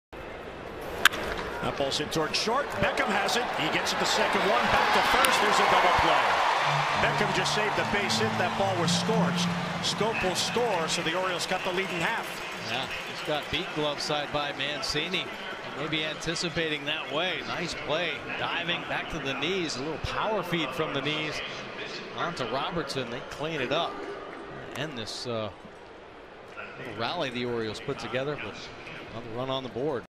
BAL@TB: Schoop scores on Castillo's double play
Welington Castillo grounds into a double play but Jonathan Schoop scores to cut the Orioles' deficit in half in the top of the 2nd